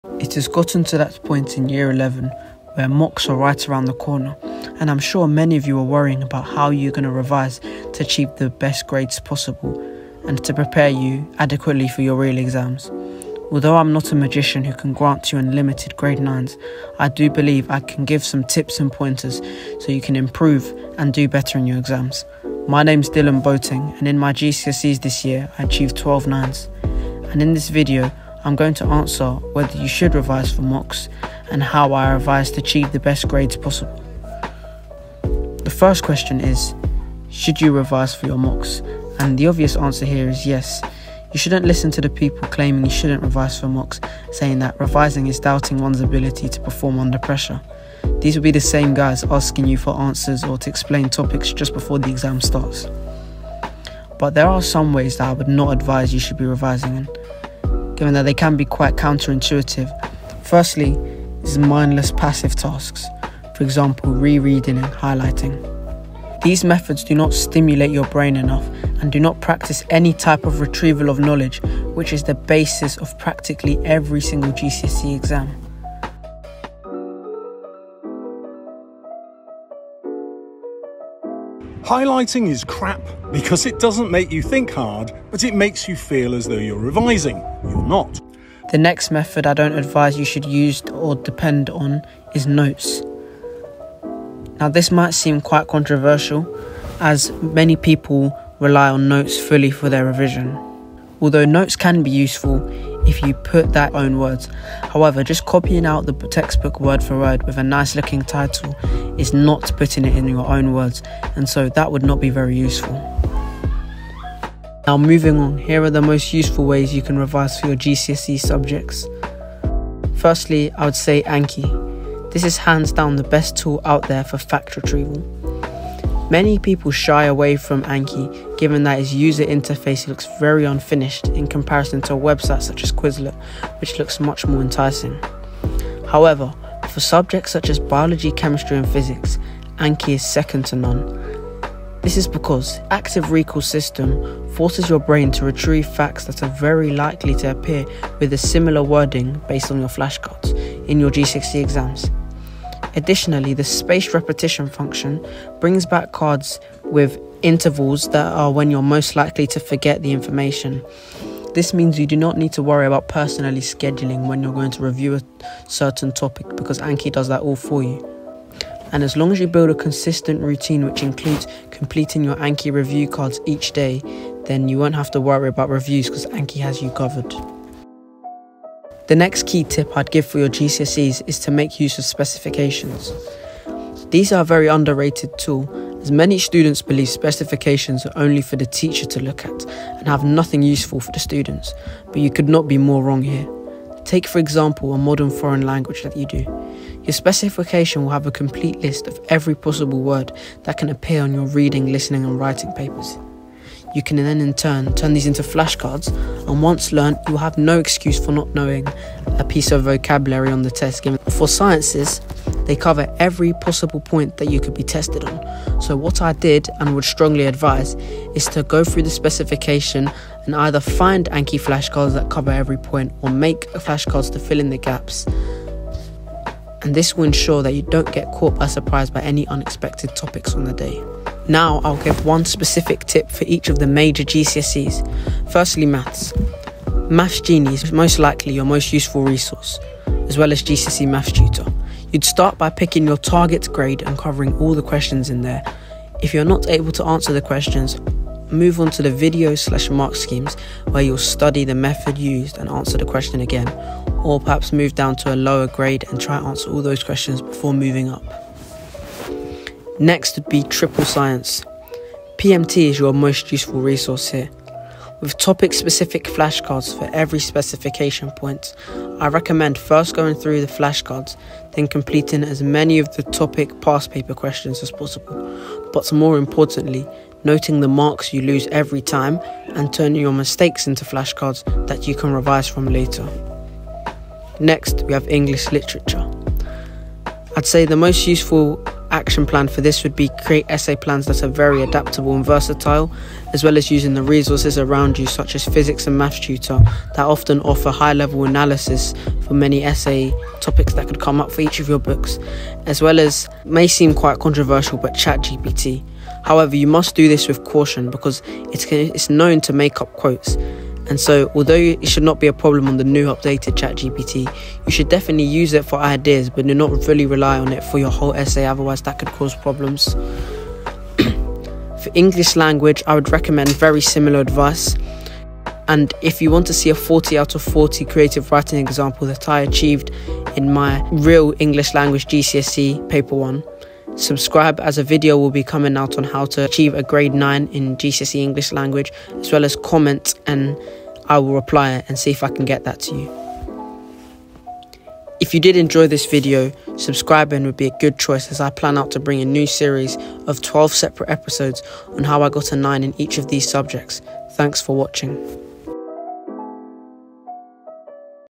GCSE MOCKS Revision Guide | How to Ace Mocks (No Nonsense Guide)
Many Year 11's are worried about their mocks that are soon approaching, and how they can best utilise this half term to revise - This video is definitely a great place to start  -

Here are the websites and apps mentioned:
Physics and Maths Tutor
Maths Genie
Anki Web
Quizlet
Subject Specific Specification can be found by typing your 'subject' - then the 'exam board' - then 'specification'

Dylan Boateng